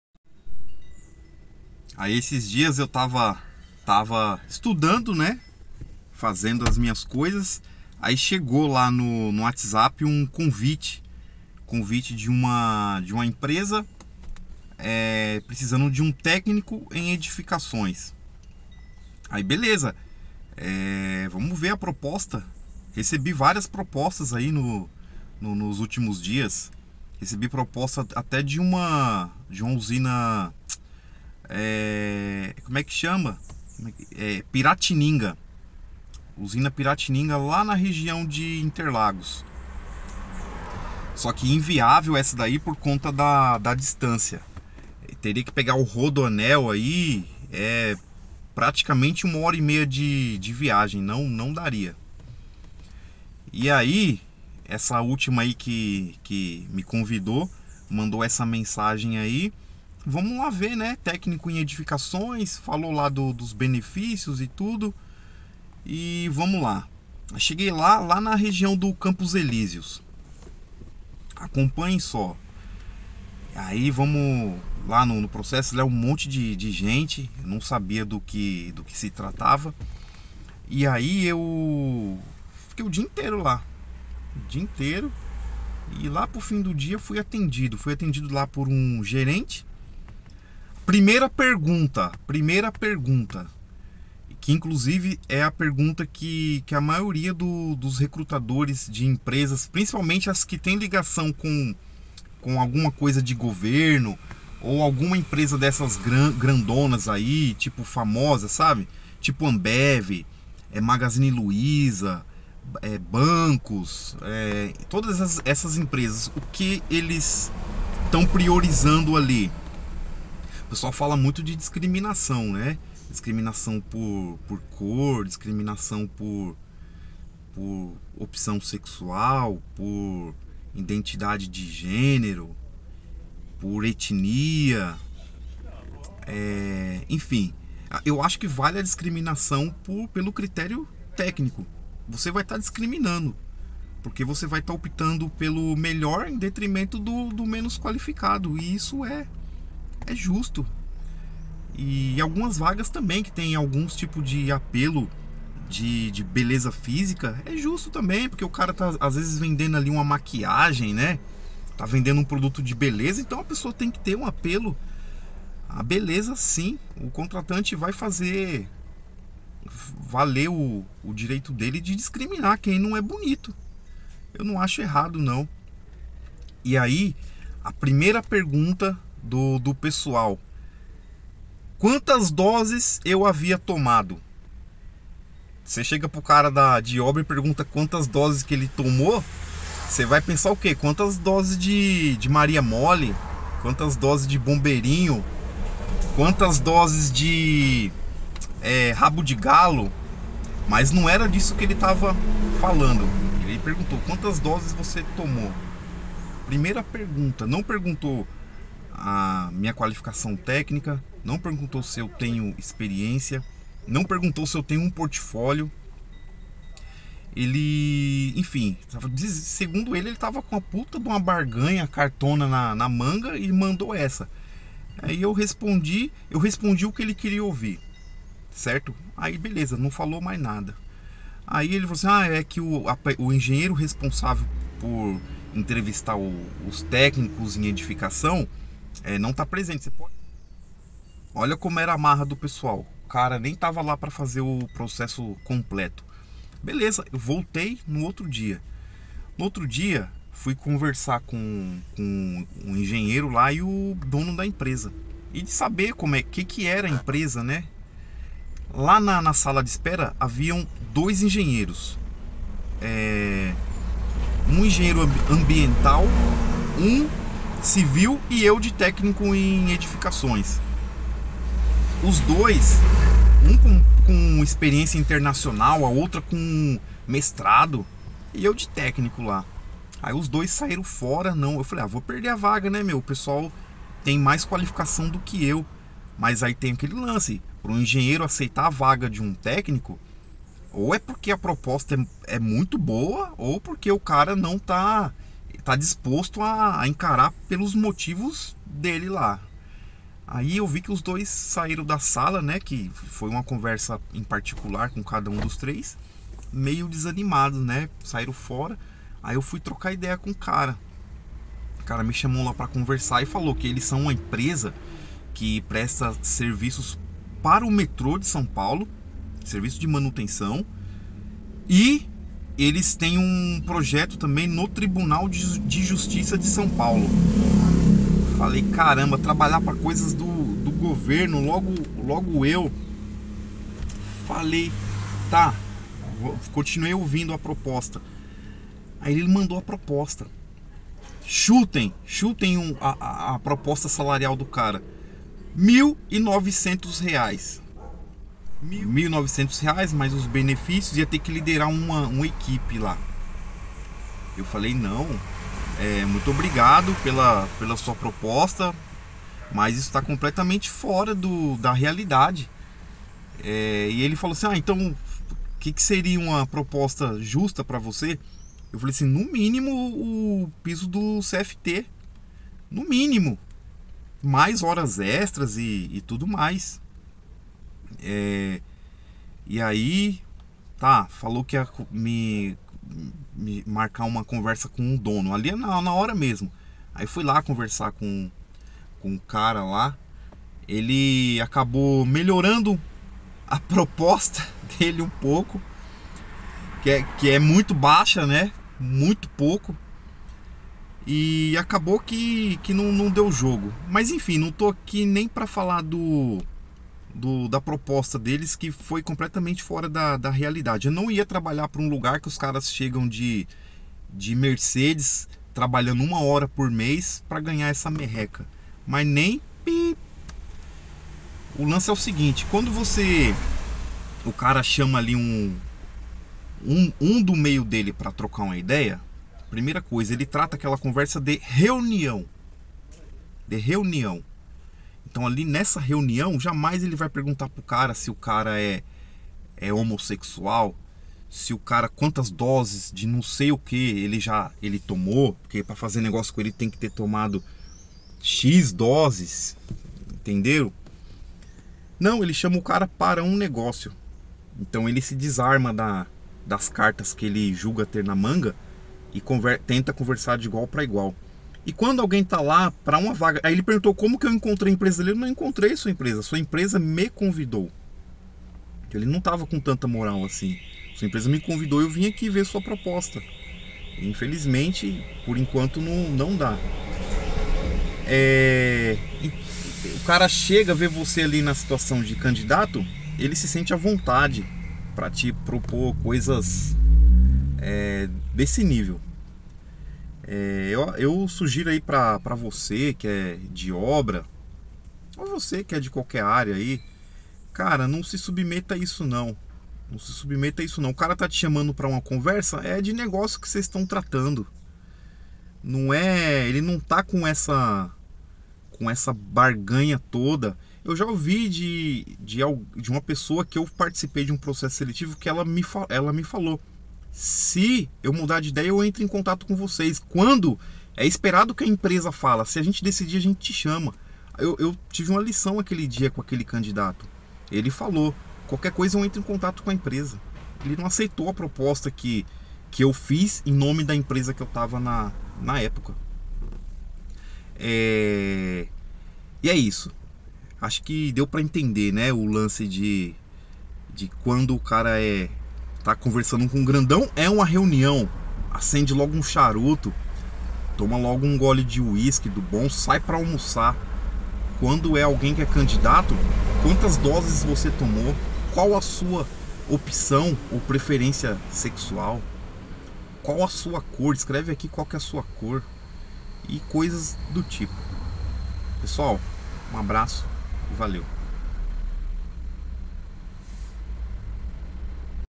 Como age alguns RHs - Entrevista de emprego
No vídeo falo sobre uma experiência com um RH de uma empresa durante um processo de contratação.